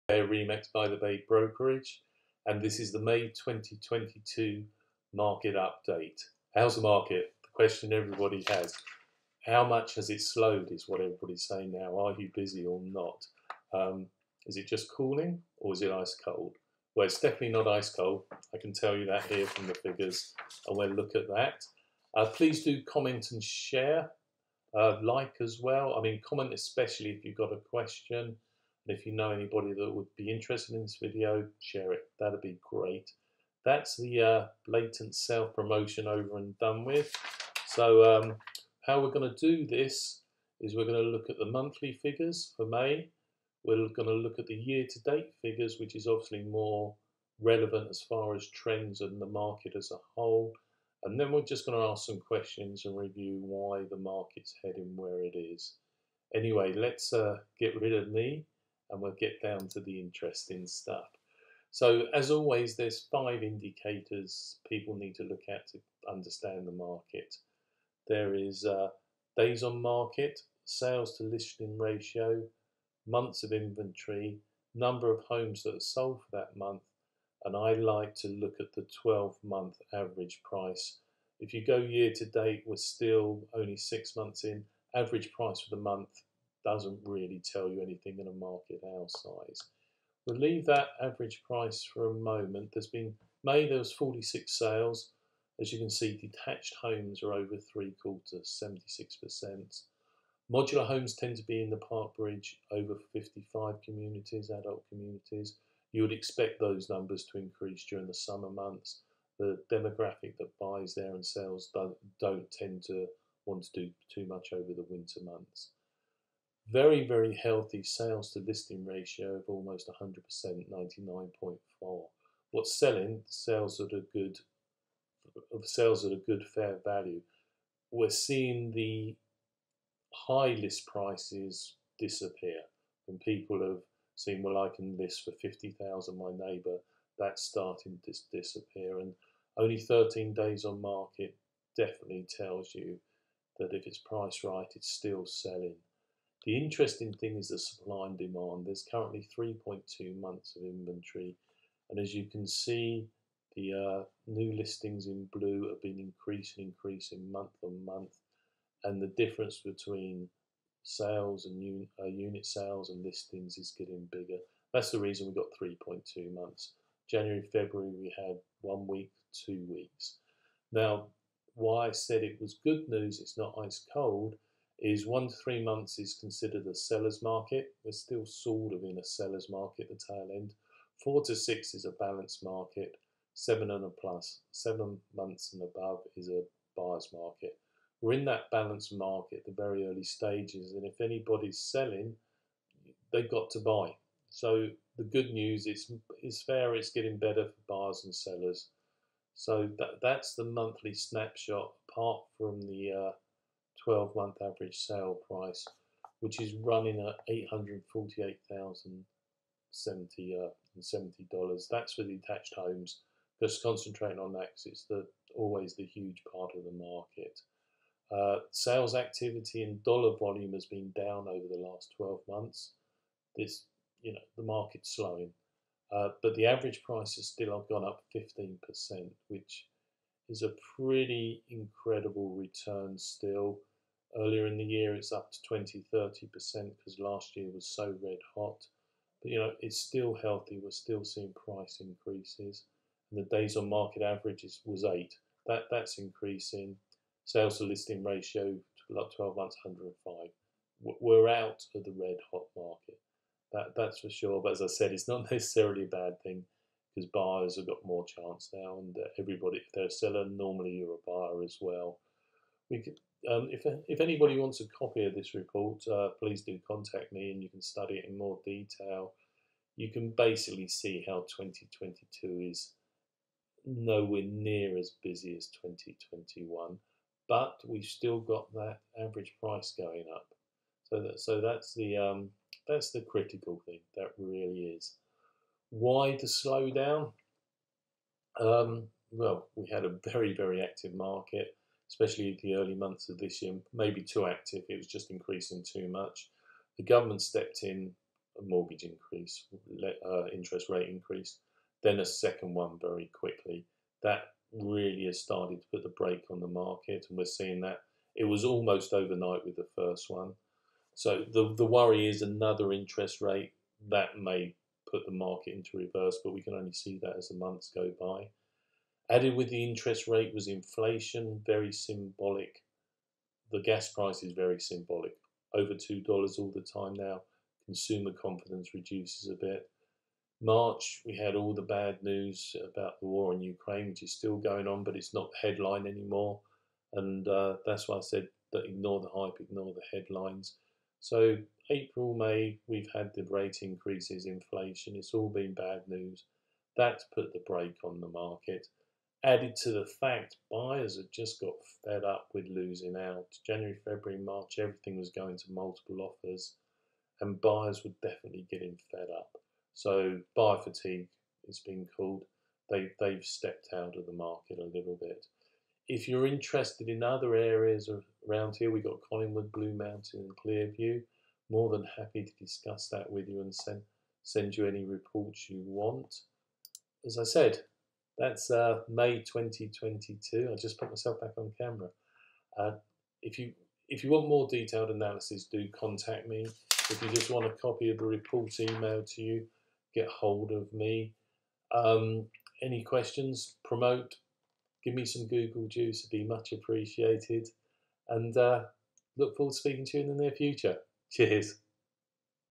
May 2022 Market Update
Andrew dives into the Simcoe county Market Update and shares what's happening in and around Georgian Bay with the Real Estate Market.